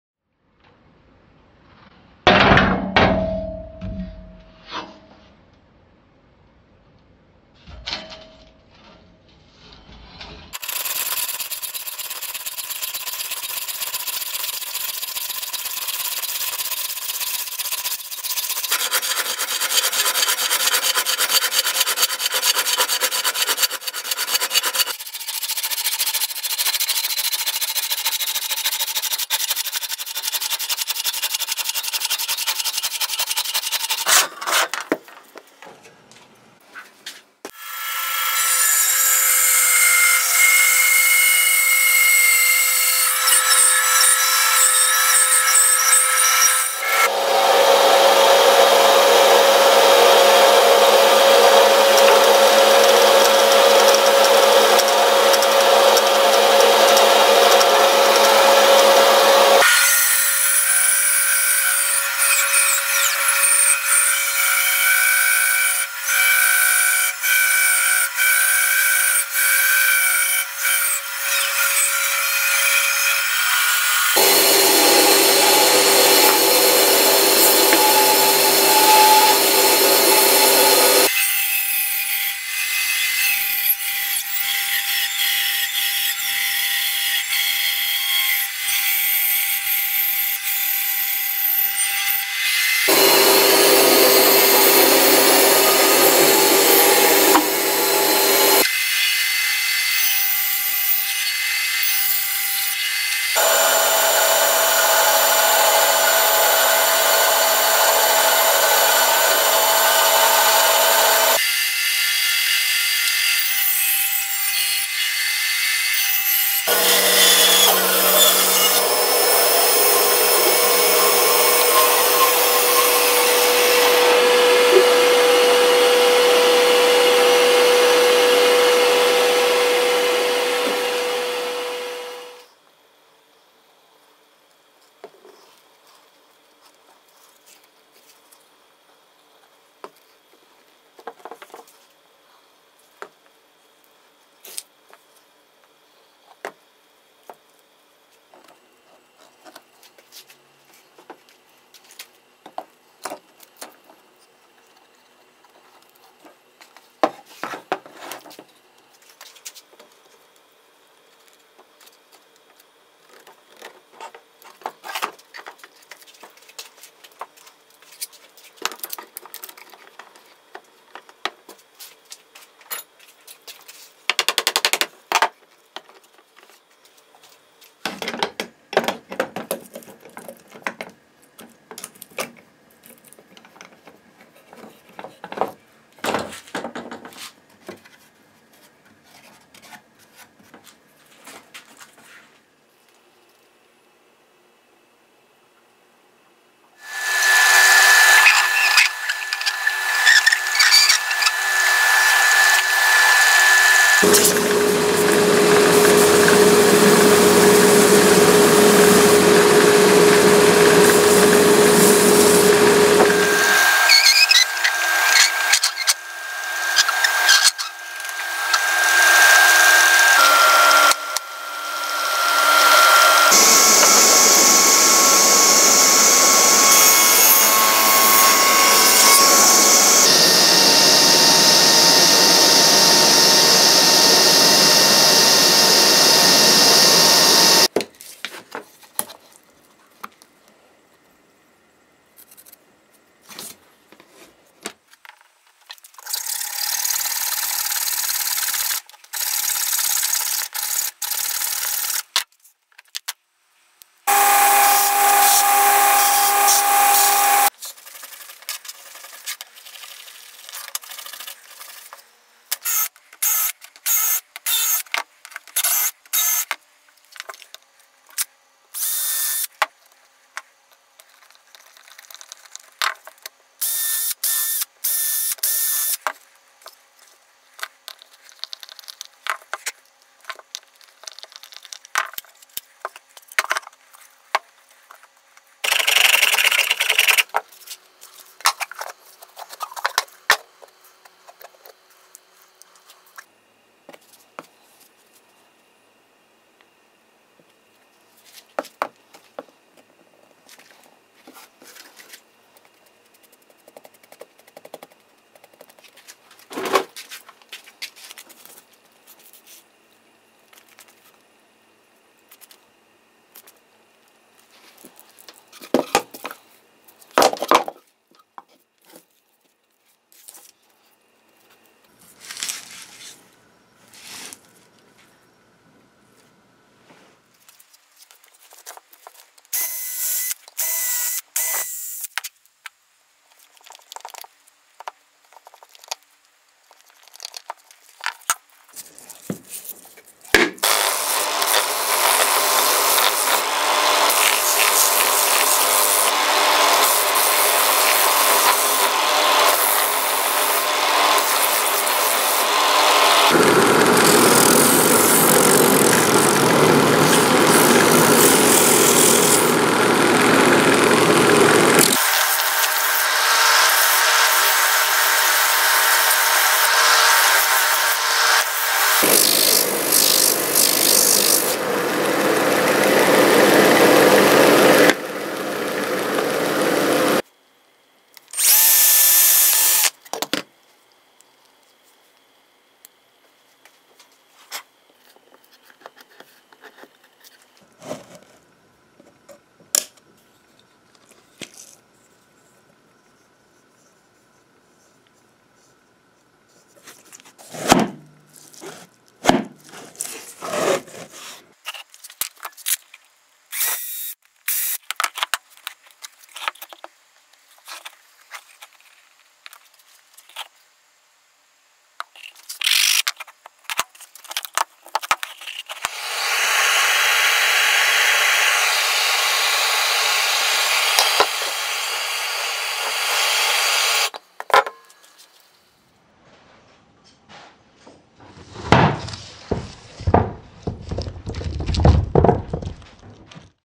DiResta: Mini Log Chest
Recently, I created the quite large "Hollow Log Treasure Chest." While I was working on this, I was envisioning the method to making a smaller version on the bandsaw. Here, you see me make the Mini Log Chest. I had to work within the limits of the band saw, with 6.25" cutting height. I kept it simple on the outside to use a secret hiding spot. Thank you, and enjoy!

Buy his bestselling book, Workshop Mastery with Jimmy DiResta at the Maker Shed